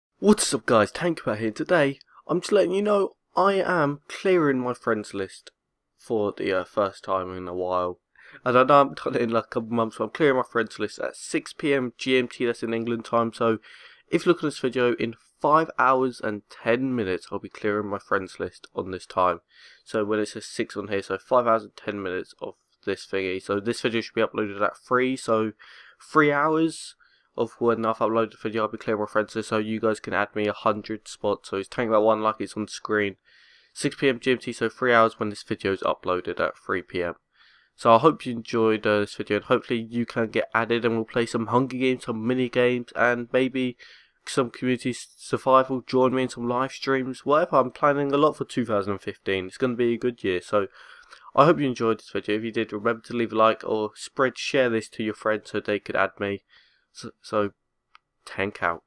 New Friends List Add Me On TankMatt1
So if you want to be able to play Minecraft Xbox with me add my subscriber account TankMatt1. I will be deleting everyone on the 1st of January at 6pm GMT.